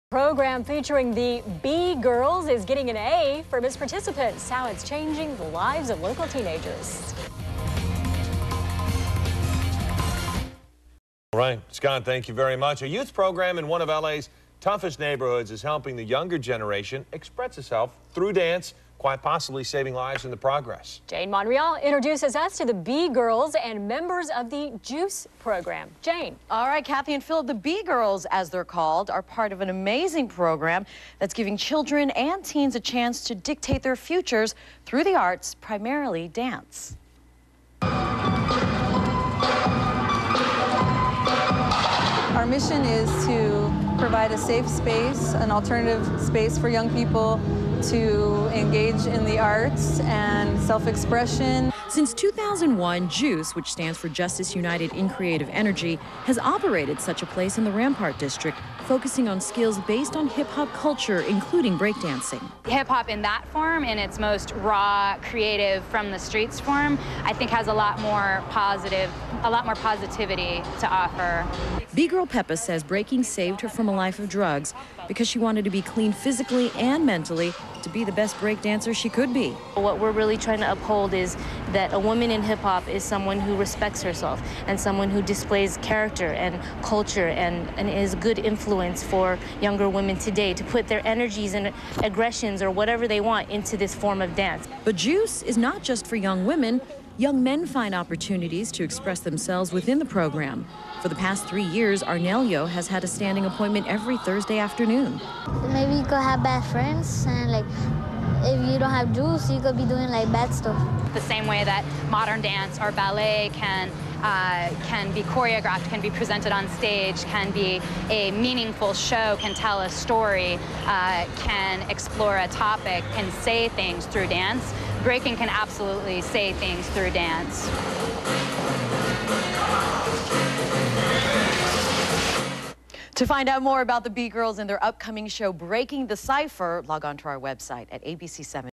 J.U.i.C.E. and Breaking the Cypher featured on ABC Channel 7
Reporter Jane Monreal interviews b-girls from Antics Performance as they rehearse for BREAKING THE CYPHER and also catches the action at J.U.i.C.E. (Justice Uniting in Creative Energy) where Los Angeles youth have the opportunity to practice breaking or learn some graffiti art skills in a positive environment.